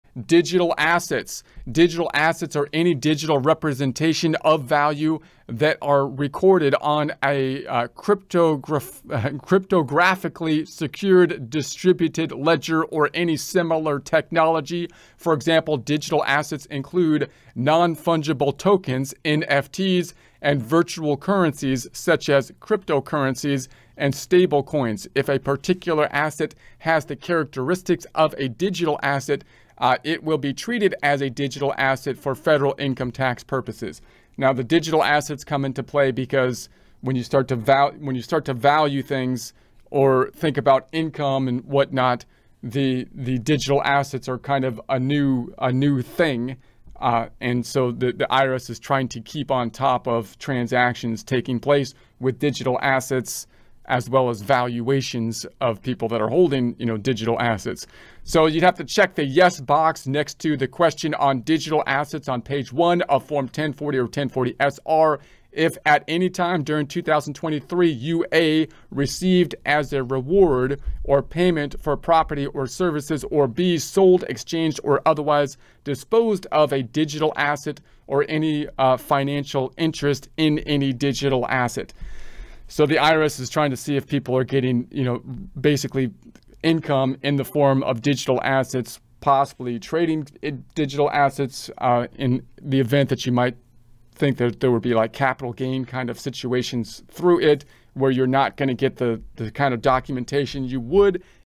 Triggering events for reporting Tax 2023-2024
"Discover the key triggering events that necessitate reporting under the Tax laws of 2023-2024. Unravel the pivotal moments that prompt tax documentation and compliance in this informative series."